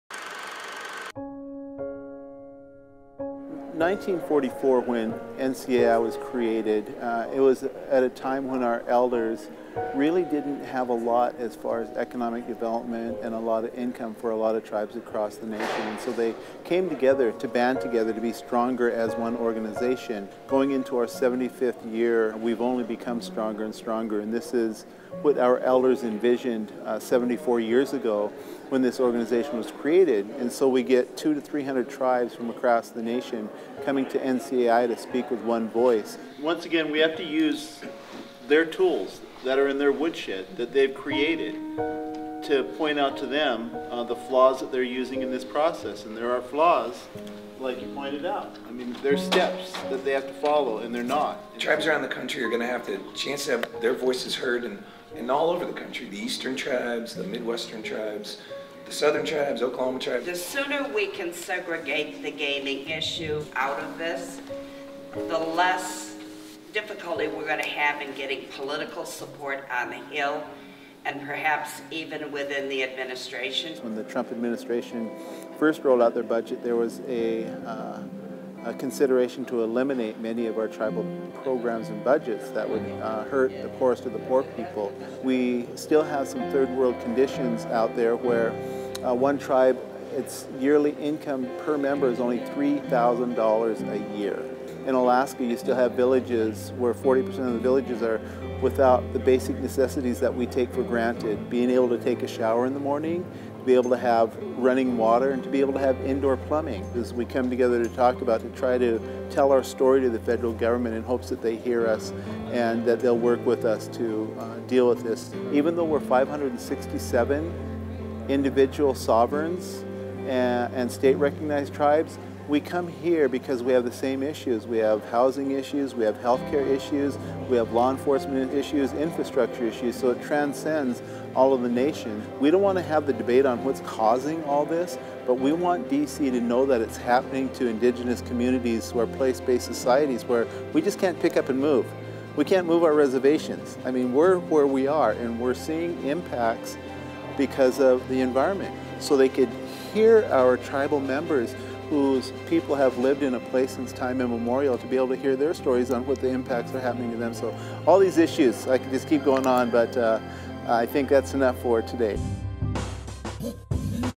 FNX Now - NCAI Story
Every year for the last 74 years, Native American tribes from all across the country have been using the National Congress of American Indians as a platform to address healthcare issues, housing issues, and infrastructure issues.